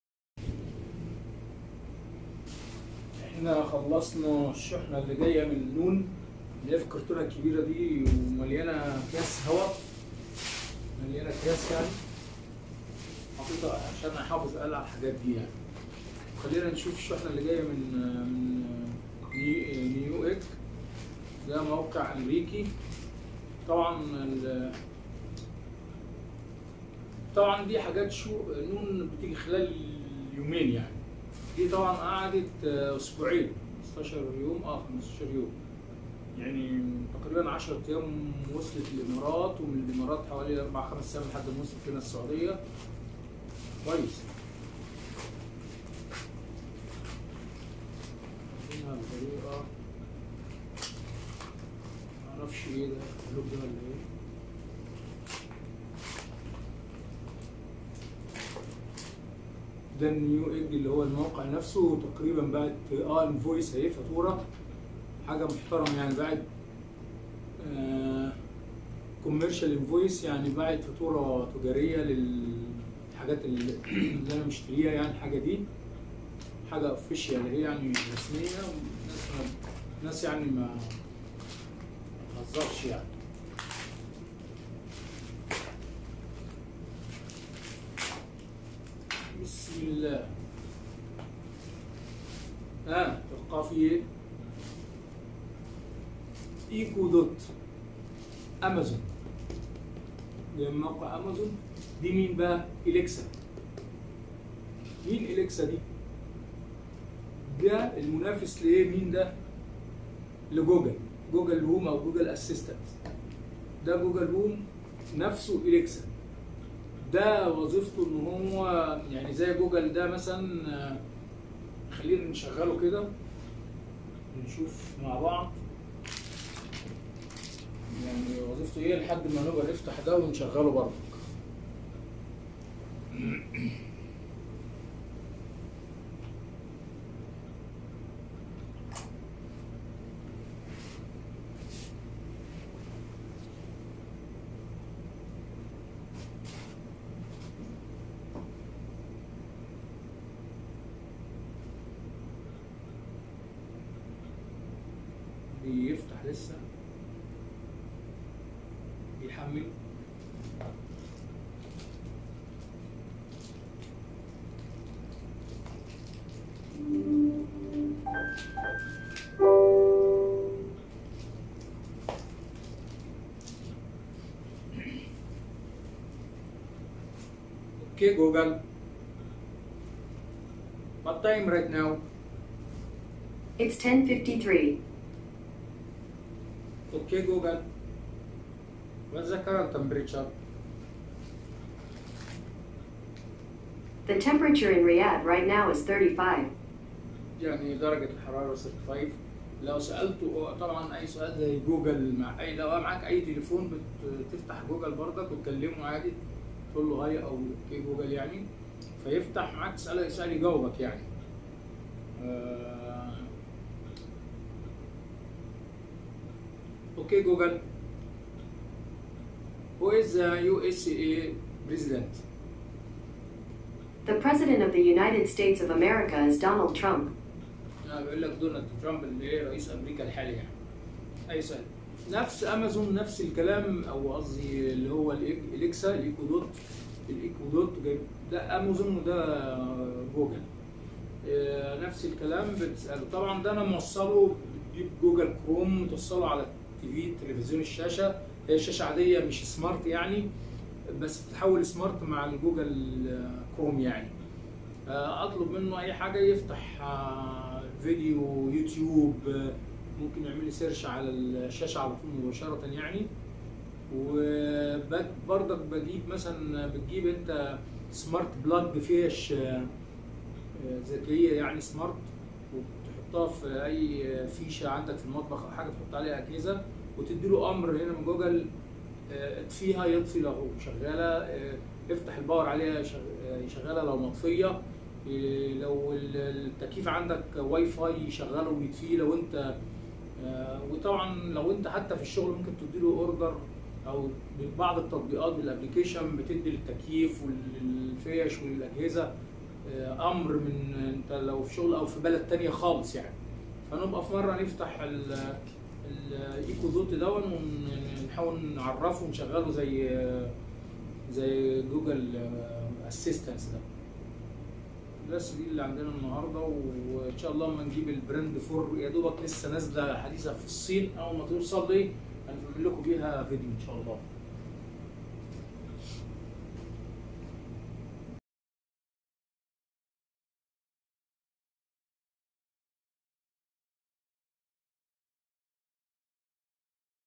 فتح شحنة من امريكا موقع newegg
فتح شحنة من امريكا موقع
 newegg
تم شراء شحنة من المريكا وصلت الشحنة في خلال اسبوعين